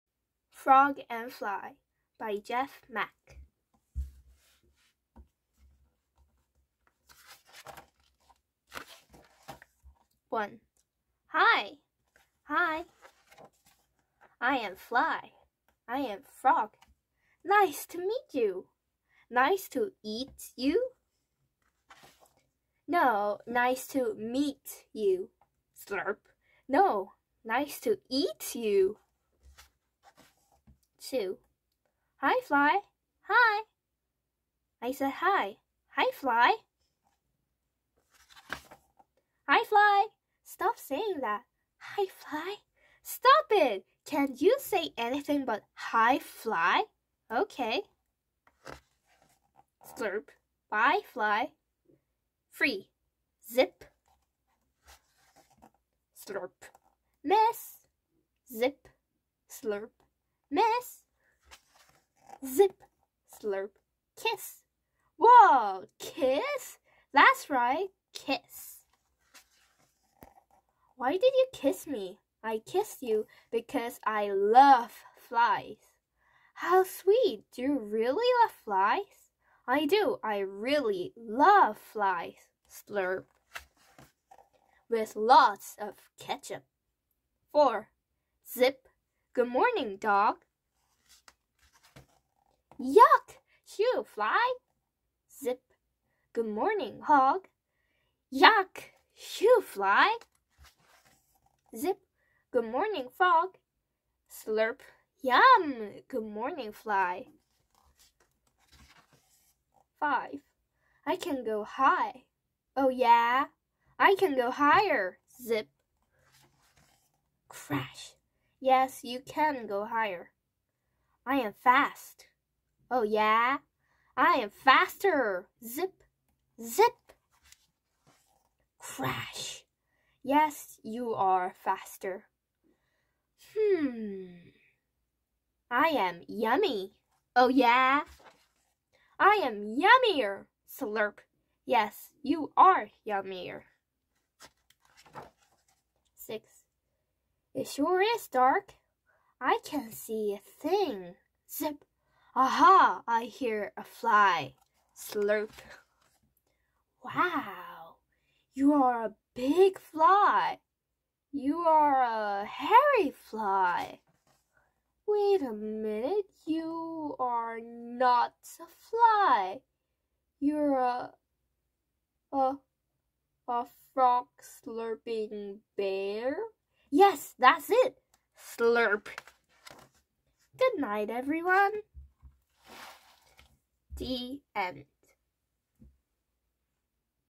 Frog and Fly
Frog and Fly read aloud
Frog and Fly by Jeff Mack
j2